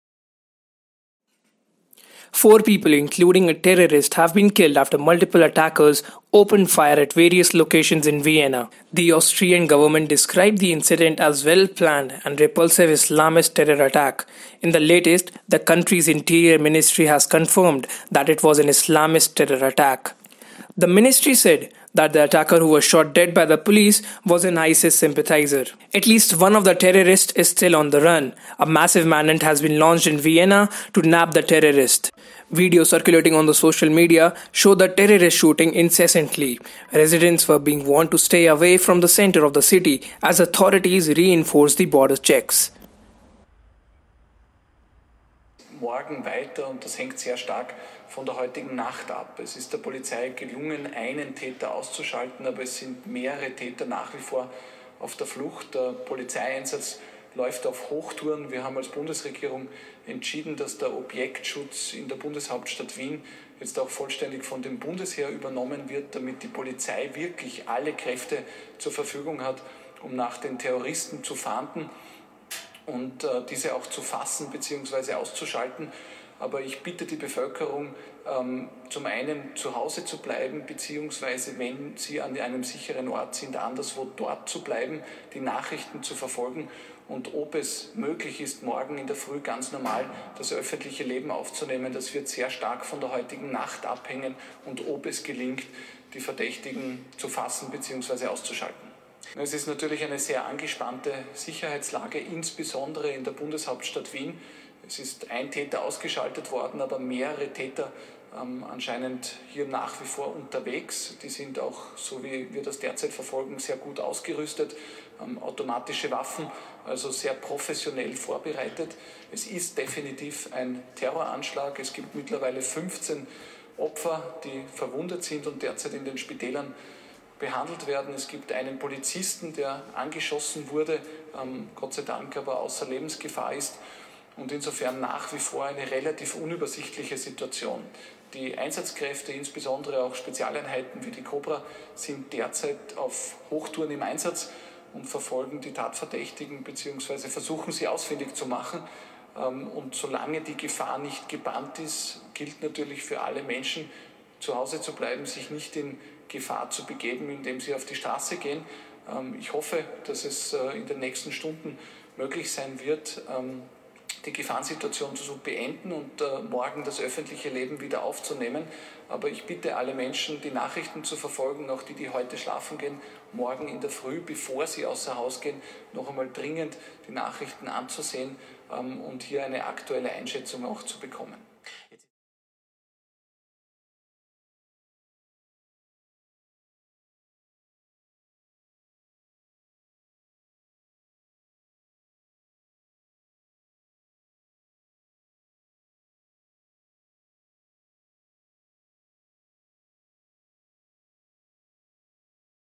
Vienna terror attack by Islamic State sympathizer shatters an evening of revelry - News 360 Tv
Vienna Islamist terror attack : At list five people, including a terrorist have been killed after multiple attackers opened fire at various locations in Vienna . The Austrian government describe the incident as well planned and repulsive Islamist terror attack, in the latest the country's interior ministry has confirmed that it was an Islamist terror attack.
The ministry said that the attacker who was shot dead by the police, was an isis sympathizer. At least one of the terrorists is still on the run, a massive manhunt has been launched in Vienna to nab the terrorist.
Video circulating on social media show the terrorist shooting incessantly, residents were being warned to stay away from the centre of the city as authorities reinforced border checks.
The gunman whom police shot had been "radicalized" and felt "closely connected" to the Islamic State group, Austria's interior minister Karl Nehammer told a news conference.
He did not give further details of the attacker's identity, but was quoted by ANA as saying the man was 20, had Macedonian roots and was known to authorities.
According to their initial assessment there were six locations targeted, but on Tuesday Nehammer said that investigators were still making assessments based on witness statements.
On Tuesday, authorities said that a suspect they had killed was an Islamic State sympathizer who was heavily armed with an assault rifle and wore a fake suicide belt.
Investigators believe that there were more attackers, and a manhunt continued through the morning.
“It was an attack motivated by hate — hate for our core values, hate for our way of life, hate for our democracy in which all people are equal in rights and dignity,” Austrian Chancellor Sebastian Kurz said in a morning news conference.
“But it’s clear that we will not be intimidated by the terrorists,” he said. “It is a battle between civilization and barbarism, and we will fight this battle with all determination.”